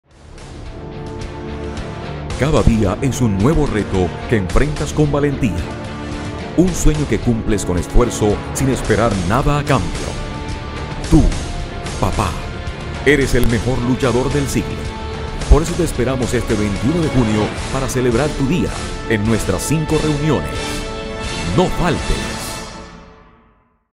El Luchador Del Siglo Día del Padre
El próximo 21 de junio se celebrará en nuestras cinco reuniones el día del padre. En este día, bajo el tema "El Luchador Del Siglo", se honrará y exaltará la gran labor que los papás desempeñan día a día para mantener una familia y ser ejemplo en todo para sus hijos.
Te invitamos a que hagas parte de esta celebración asistiendo a cualquiera de nuestra cinco reuniones dominicales: 7:00am, 9:00am, 11:00am, 5:00pm y 7:00pm.
¡Te esperamos!